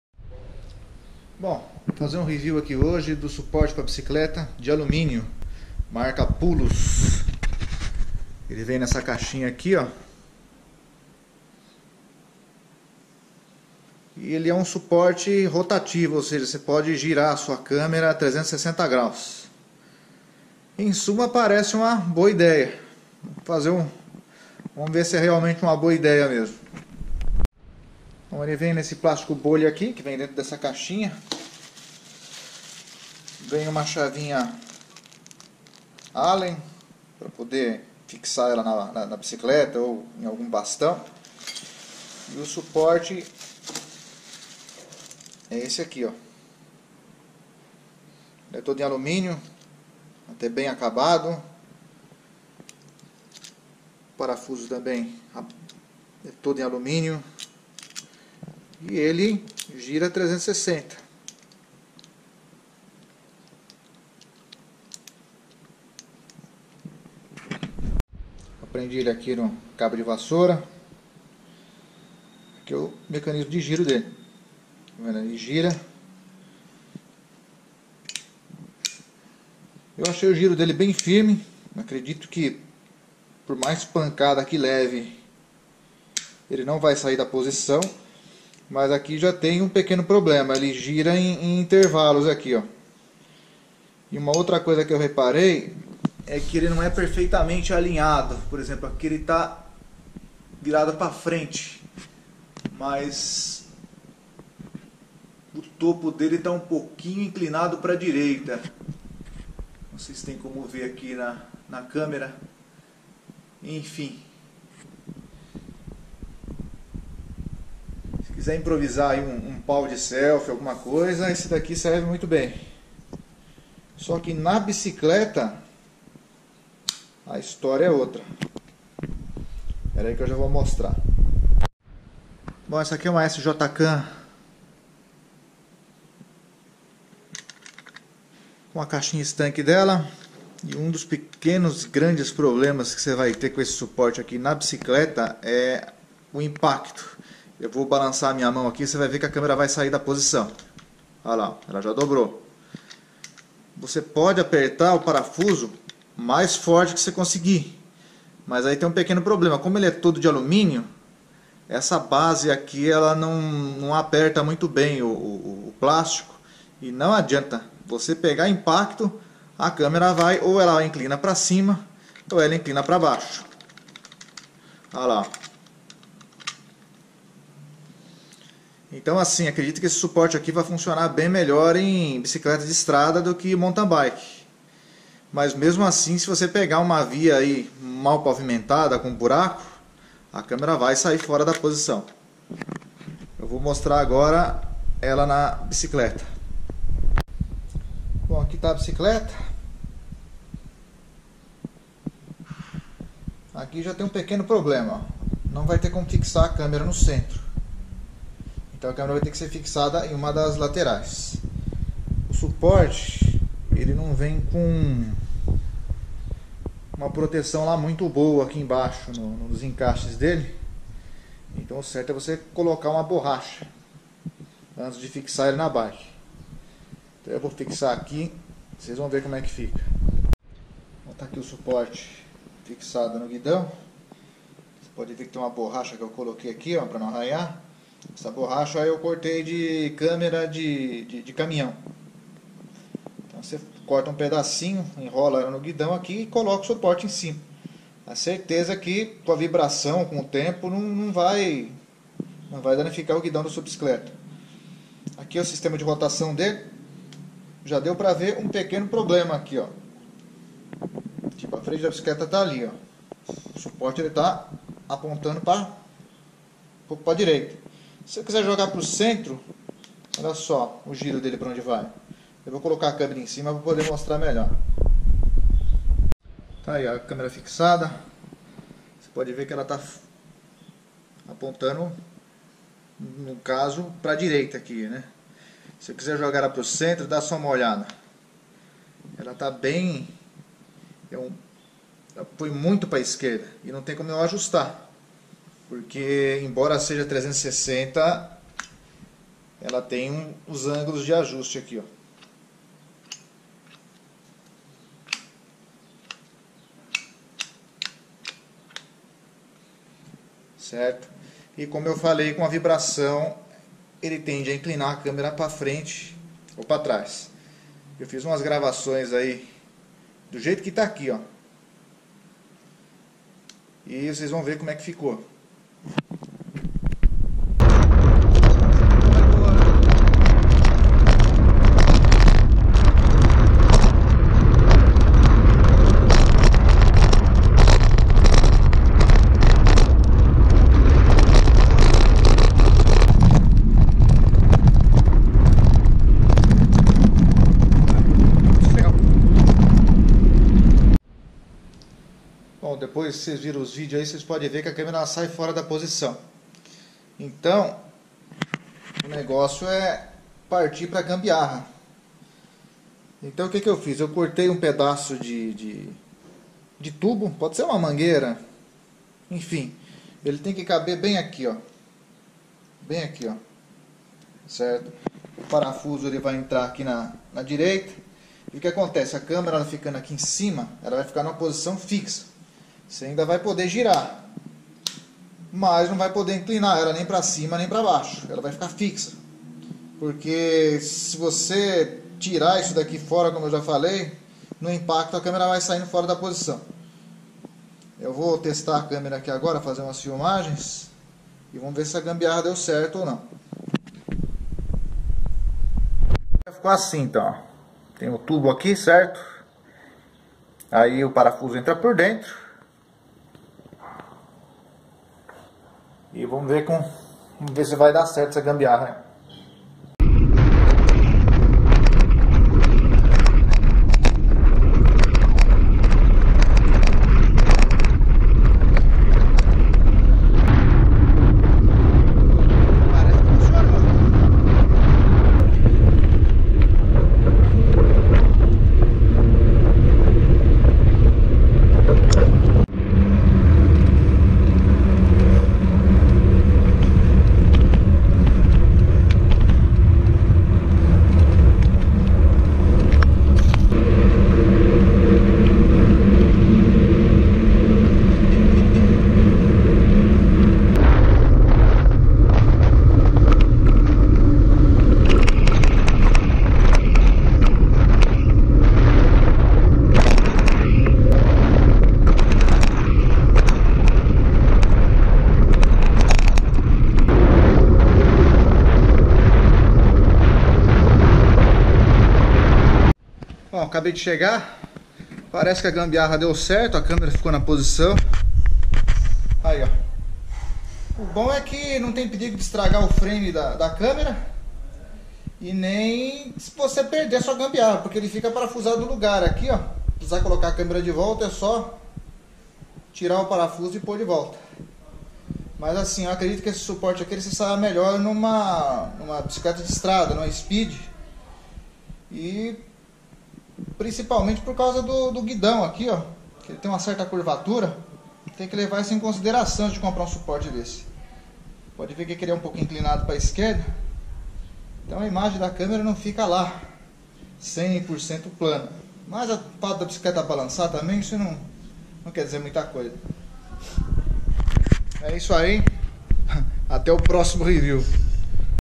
Review - Suporte de Guidão em Alumínio para GoPro Puluz 360º
Comprei no Aliexpress, paguei 10 dólares. Infelizmente não recomendo este suporte, assista ao vídeo e tire suas conclusões